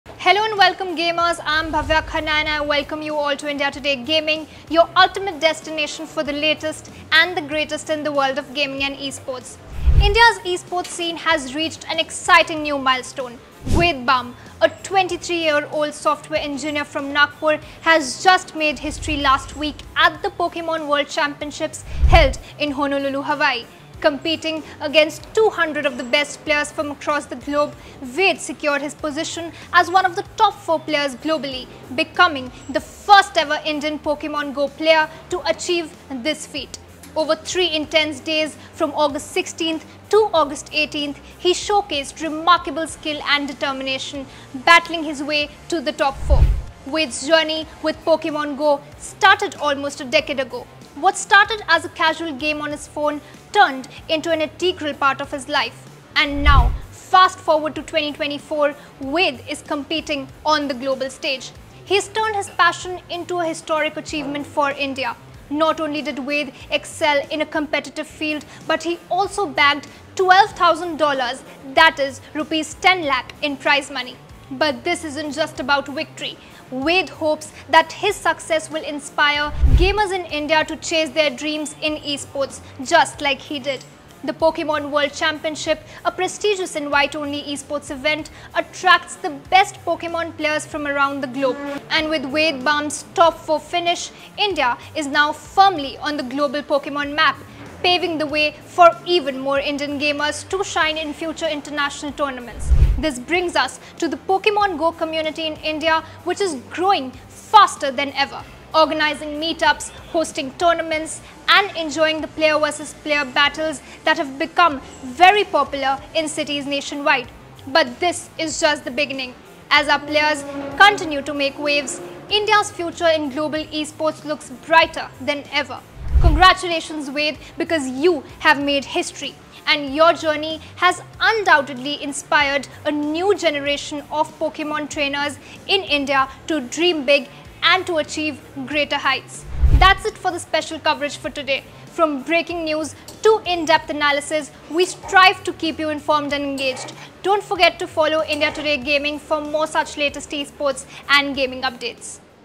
First Indian To Reach Top 4 at Pokemon World Championships I India's Pokemon Go Community
Welcome to our special coverage of esports and gaming:

Ved Bamb, a 23-year-old software engineer from Nagpur, has just made history last week at the Pokémon World Championships held in Honolulu, Hawaii. Competing against 200 of the best players from across the globe, Ved secured his position as one of the top 4 players globally —becoming the first-ever Indian Pokémon GO player to achieve this feat! Over three intense days—August 16 to Aug 18th—he showcased remarkable skill and determination, battling his way to the top four! Not only did Ved excel in a competitive field, but he also bagged 12,000 dollars that is rupees 10 lakh in prize money! But this isn't just about victory.The Pokémon World Championship, a prestigious invite-only esports event, attracts the best Pokémon players from around the world. And With Ved bamb’s top-four finish, India is now firmly on the global Pokémon map, paving the way for even more Indian gamers to shine in future international tournaments. This brings us to the Pokémon GO community in India which is growing faster than ever-organizing meet-ups, hosting tournaments, and enjoying the Player vs Player (PvP) battles that have become popular in cities nationwide. But this is just the beginning! As our players continues to make waves, India’s future in global esports looks brighter than ever. Congratulations, Ved—you have made history and your journey has undoubtedly inspired a new generation of Pokémon Trainers in India to dream big and achieve greater heights! That’s it for the special coverage of today.